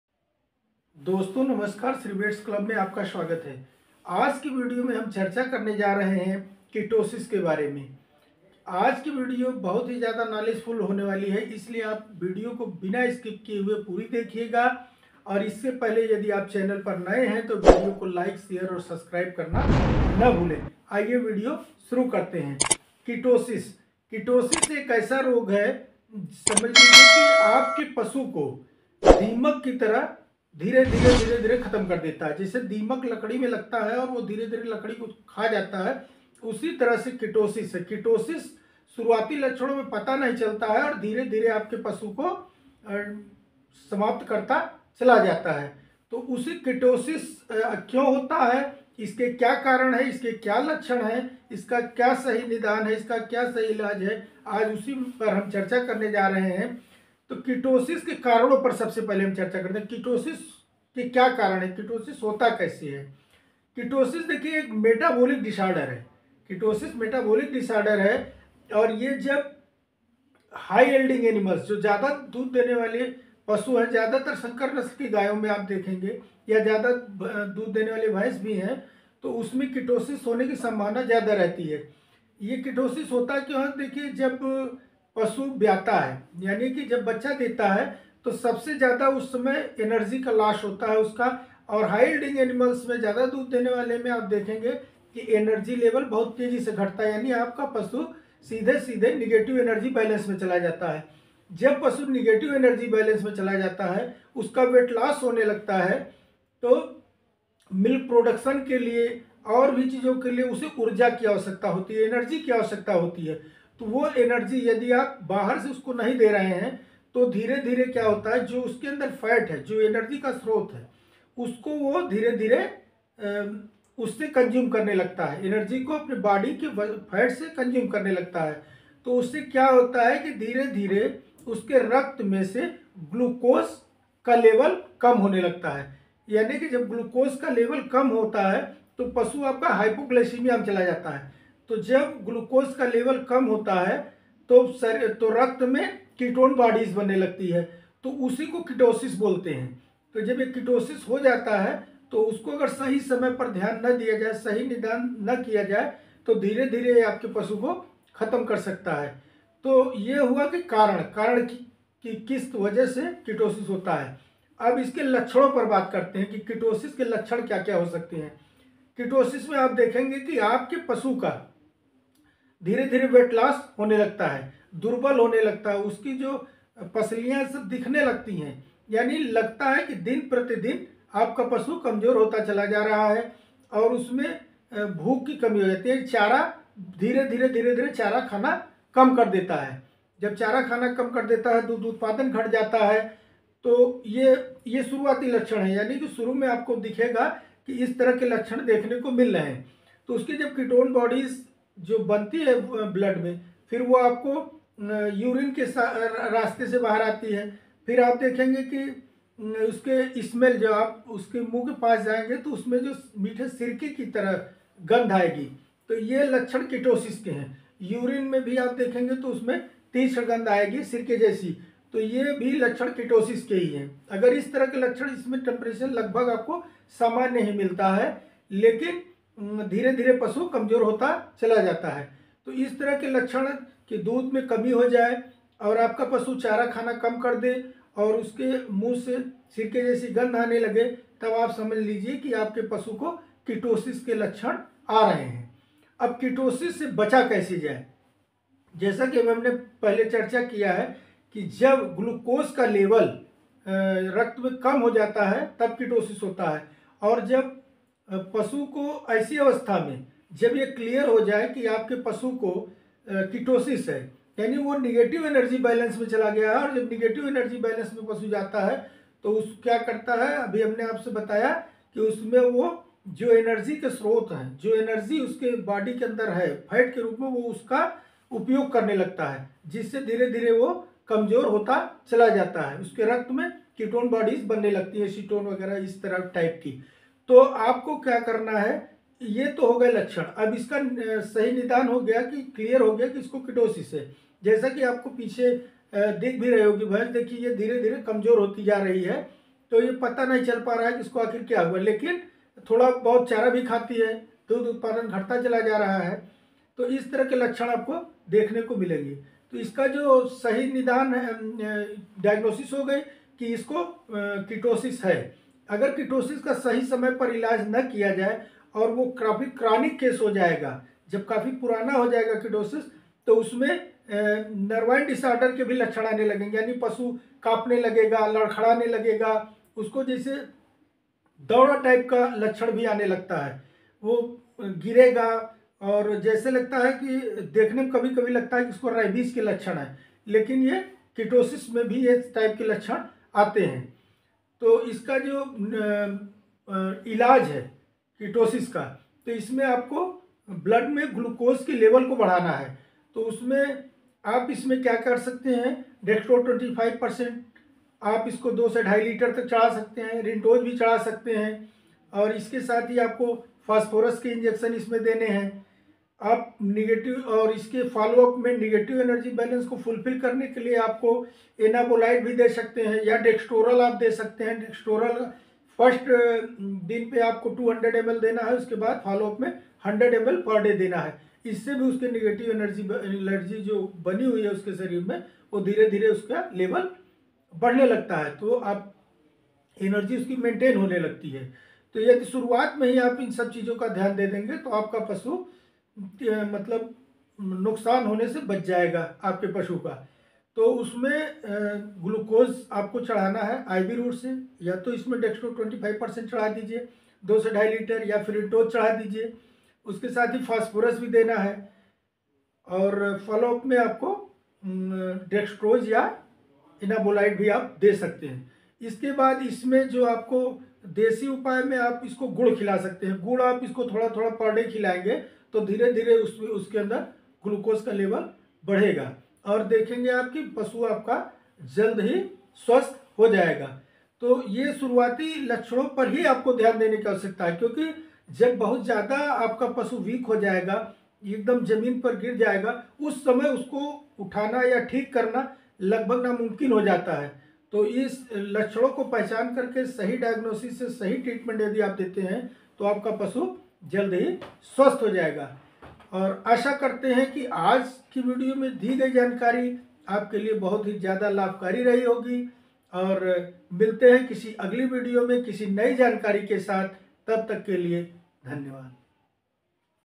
Ketosis | गाय,भैंस में किटोसिस | treatment of ketosis
किटोसिस एक मेटाबोलिक डिसार्डर है, रक्त में ग्लुकोज का लेवल गिर जाने से यह रोग होता हैI
सही निदान के साथ सही इलाज करें पशुचिकित्सक की सलाह अवश्य लें धन्यवाद।